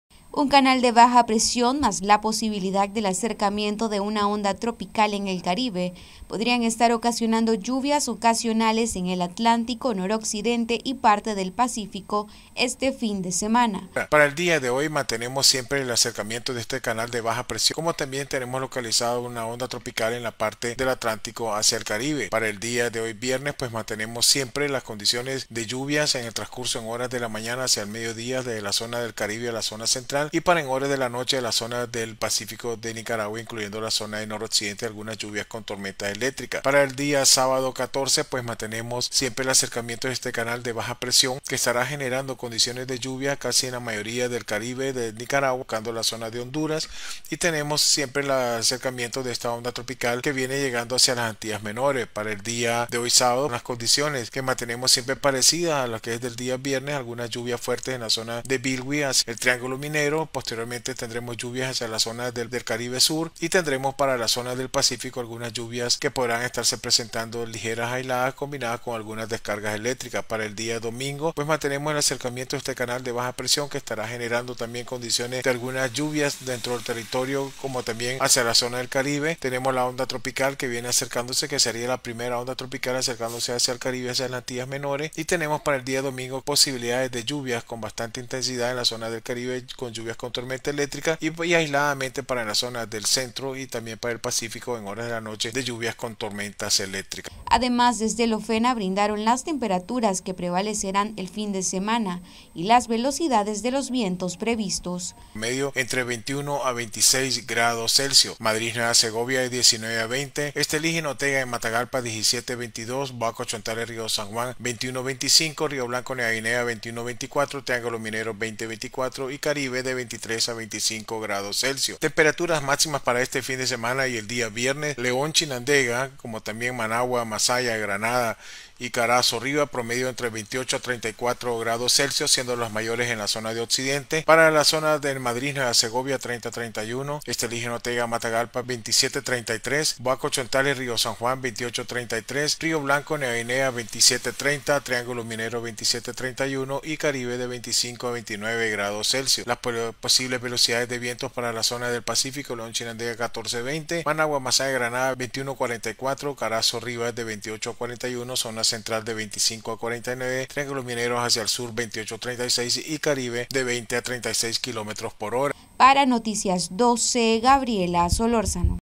Conozca las condiciones climáticas esperadas para el fin de semana en el territorio nacional
Noticias12Nicaragua Conozca las condiciones climáticas esperadas para el fin de semana en el territorio nacional.